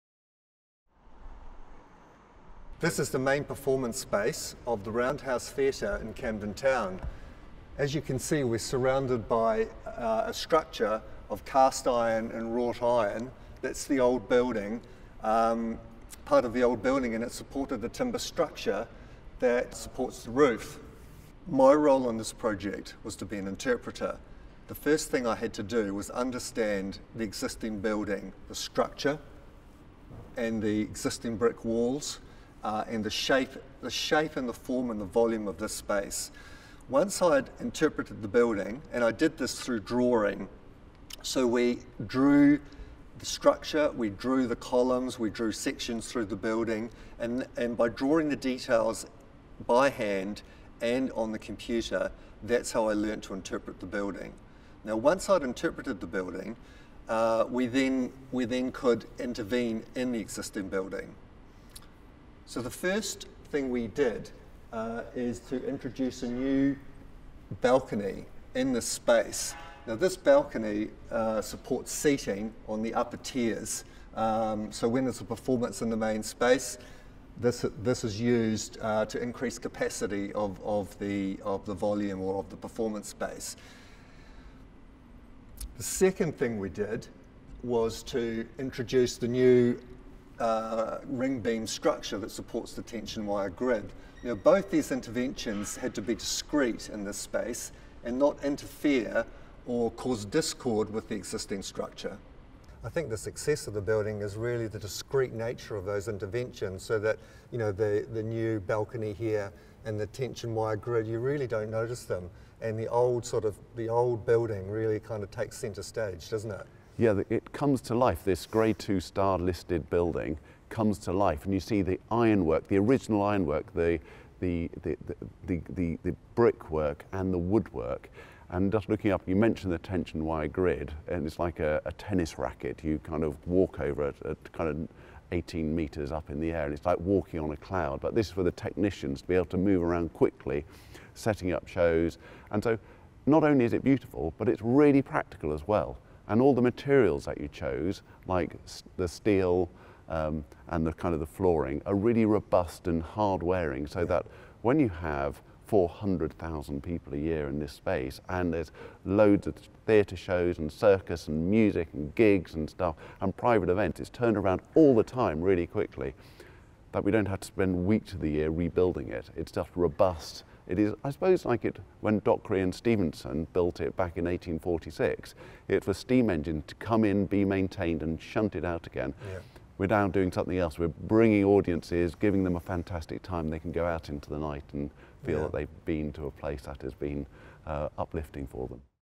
Camden Roundhouse Project - Graham Ford discusses with Marcus Davey
Graham Ford discusses The Roundhouse project with Marcus Davey, The Roundhouse Chief Executive.